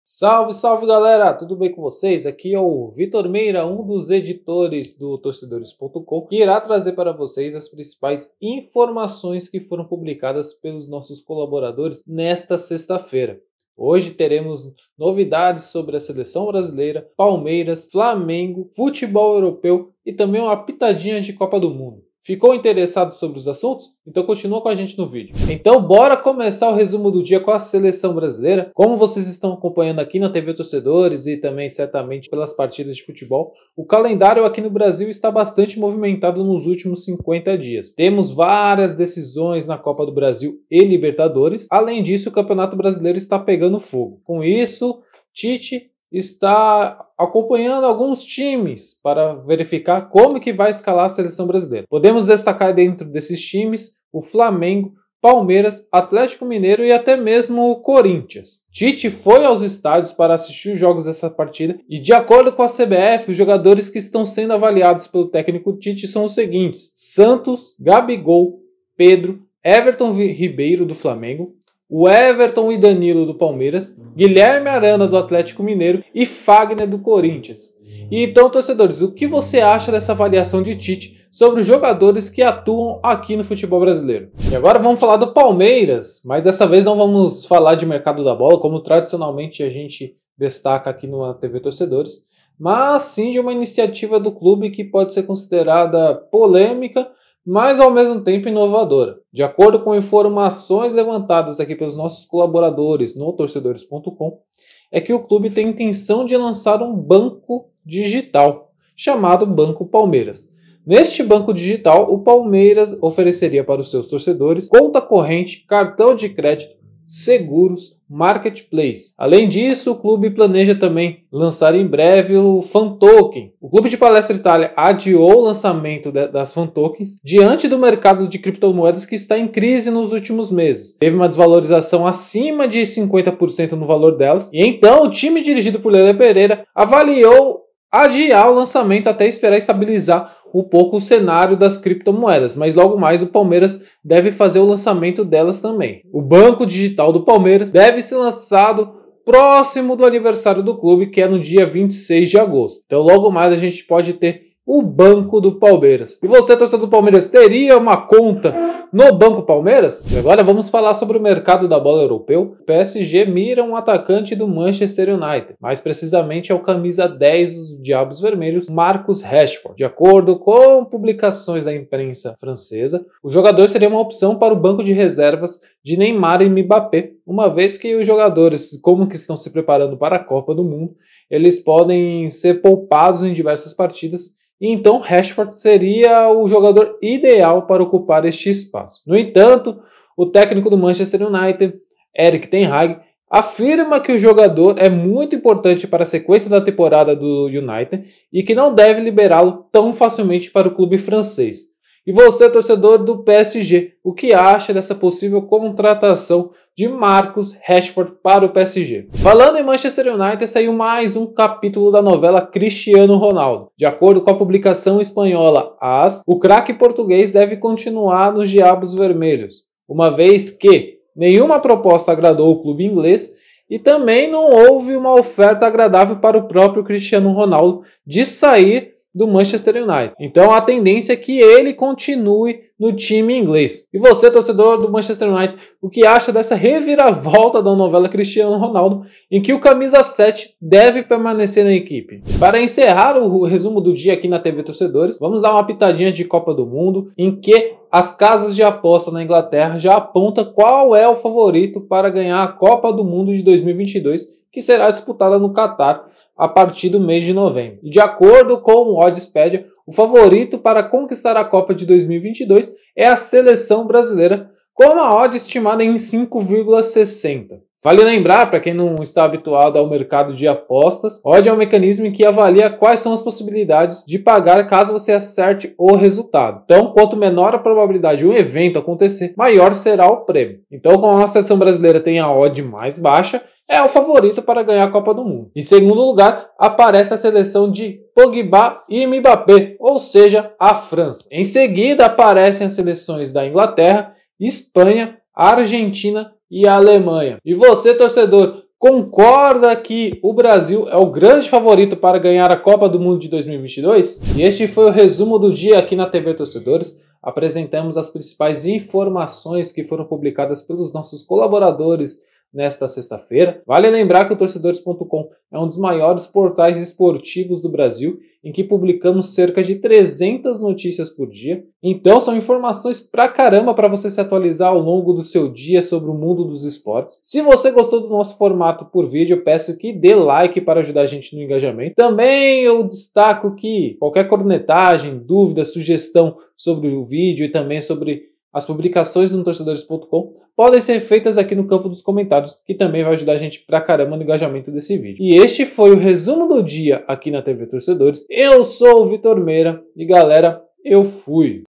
PSG mira atacante do Manchester United; CR7 define situação; favoritos da Copa do Mundo 2022
A TV Torcedores apresenta as principais informações publicadas pelos colaboradores nesta sexta-feira (12). No resumo, há notícias sobre o Tite avaliando os jogadores que atuam no Brasil para a seleção brasileira; Palmeiras planeja o lançamento de um banco para os torcedores; PSG mira atacante do Manchester United; Cristiano Ronaldo define a sua situação; e casas de apostas apontam os favoritos para a Copa do Mundo 2022.